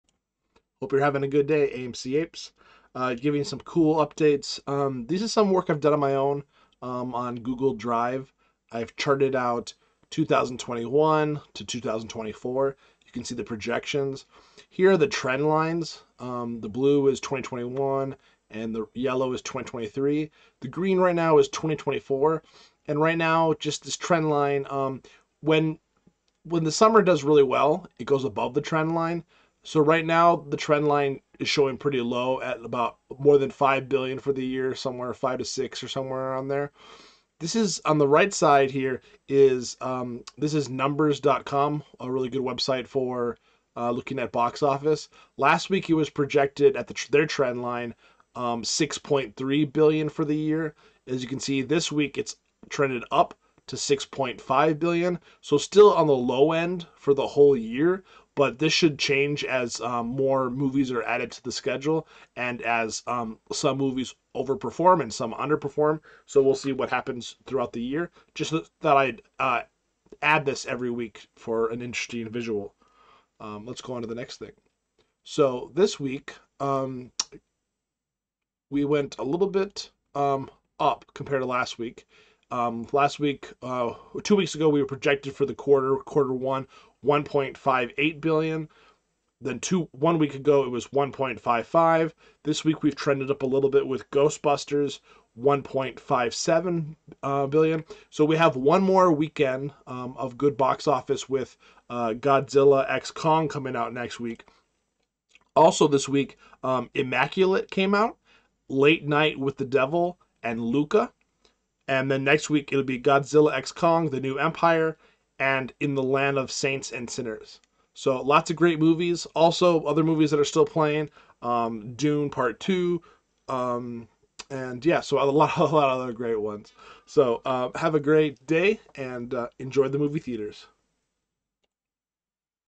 AMC Movie Theaters ~ Quarter 1 2024 Domestic Box Office Projections Based on March 24th, 2024
I am an APE, an AMC Movie Theatres Investor, and I am passionate about the Movie Theatre Experience. Just sharing some thoughts and projections on what I currently see in the data. I will do future updates throughout Quarter 1 2024, as data comes out.